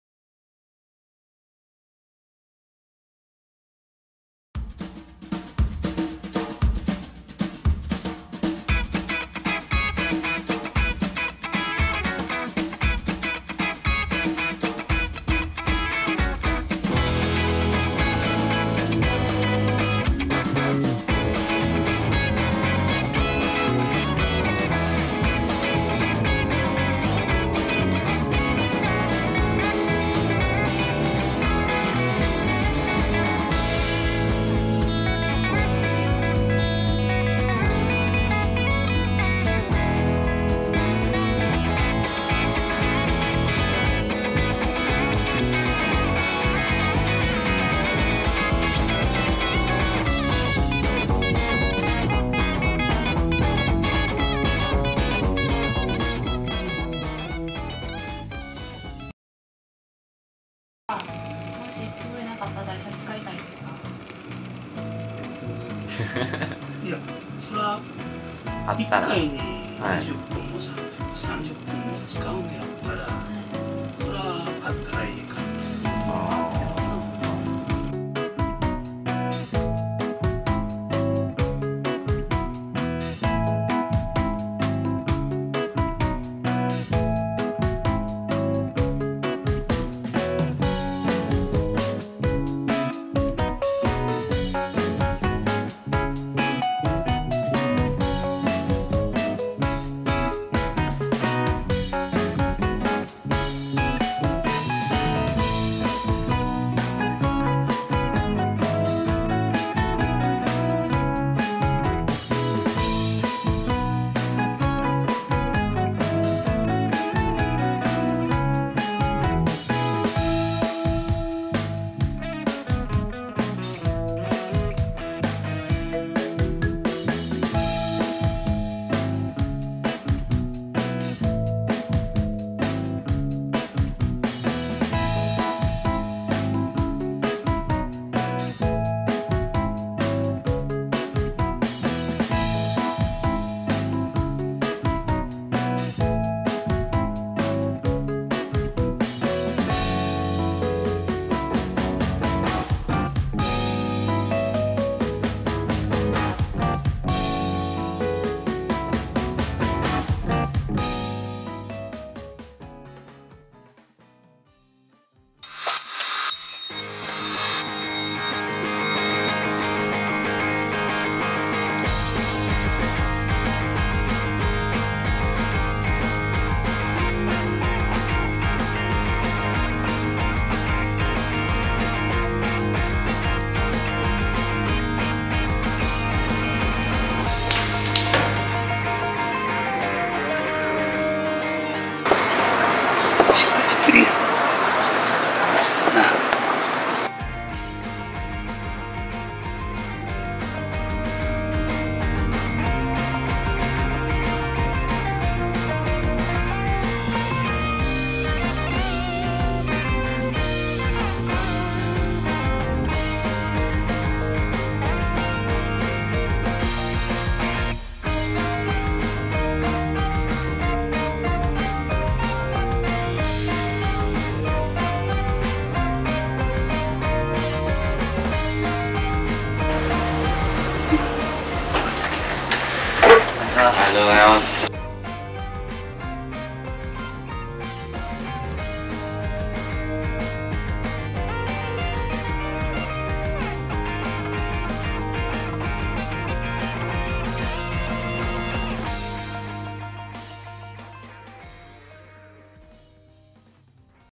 Rice Trolley for Mr. Kitagawa
Mr. Kitagawa works in a shop and has regular deliveries of rice. Each bag weighing 30 kg has to be carried to the back of the shop. A team from Kyoto Institute of Technology create trolley to help Mr Kitagawa.

Fixperts: Fuji Takumi, Shun Goi, Tomoki Hashizume, Kaho Bando,
Fix Partner: Kenichi Kitagawa
Fix Filmmakers: Fuji Takumi, Shun Goi, Tomoki Hashizume, Kaho Bando
Education Institute: Kyoto Lab Design